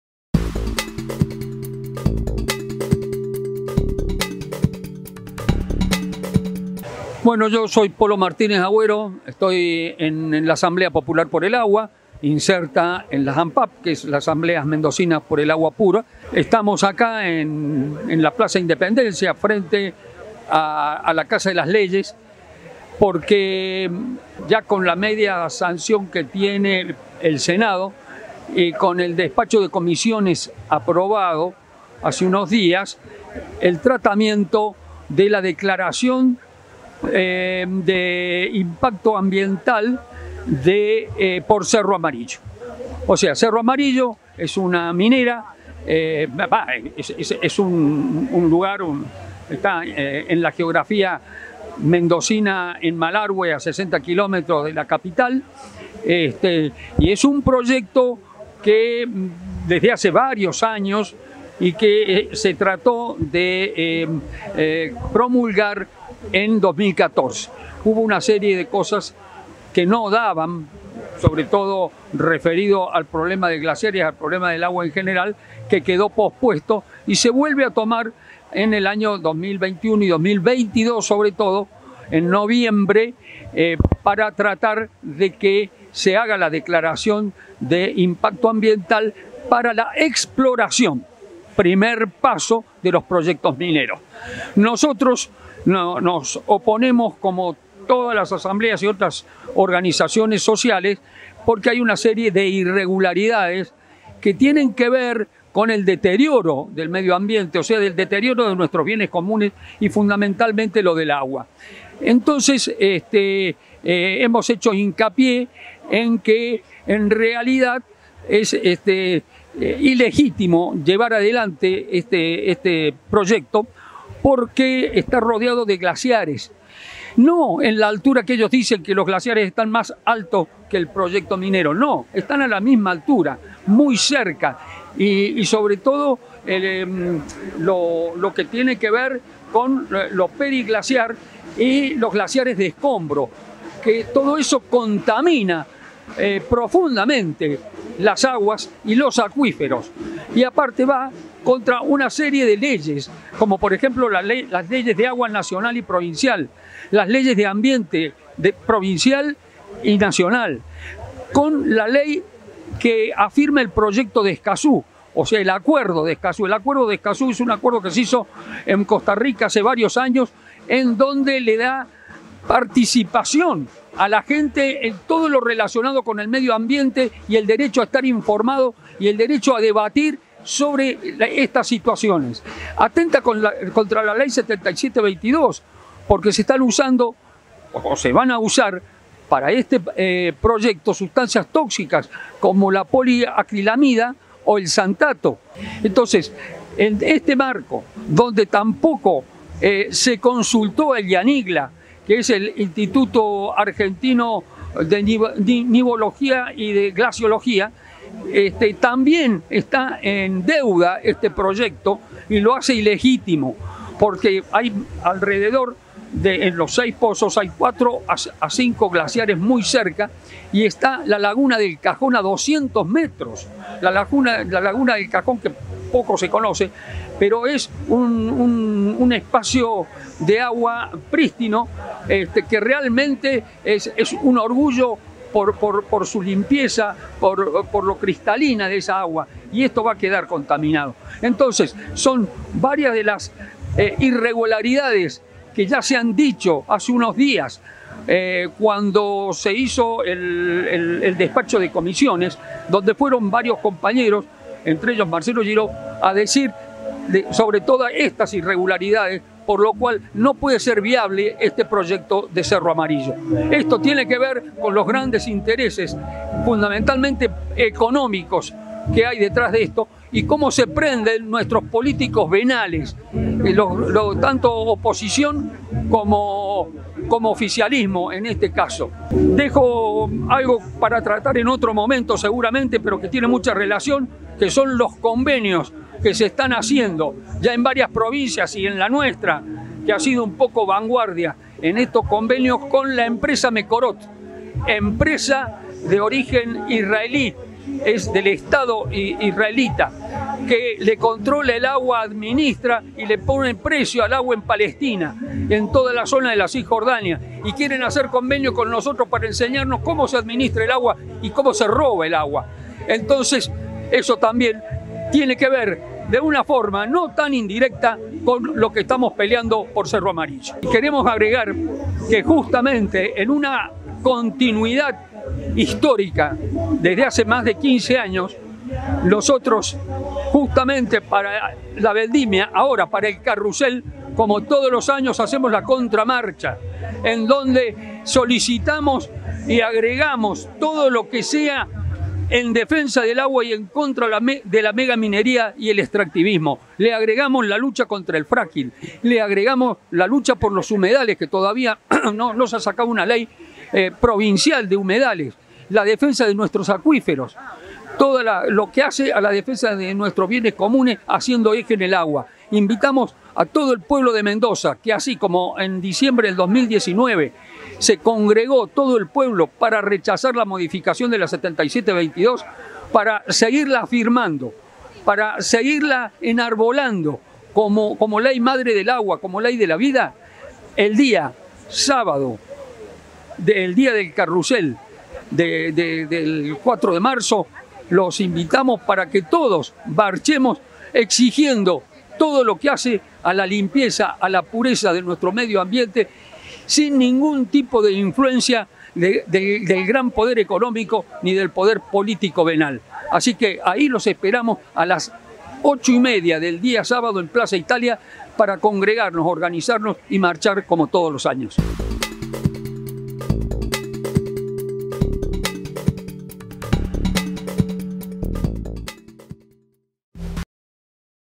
Asambleas repudiaron el tratamiento y aprobación del proyecto cerro amarillo
La Cámara de Diputados de Mendoza aprobó la declaración de impacto ambiental (DIA) para realizar la exploración de la mina de cobre Cerro Amarillo, ubicada en el departamento de Malargüe.
Durante el tratamiento, asambleas ciudadanas en defensa de los bienes comunes se convocaron para repudiar este proyecto y denunciar el incumplimiento de varias leyes y acuerdos, tanto provinciales como nacionales e internacionales.
En esta entrevista, Polo Martinez Agüero, integrante de la Asamblea popular por el agua cuenta las razones por las que este proyecto no tiene licencia social.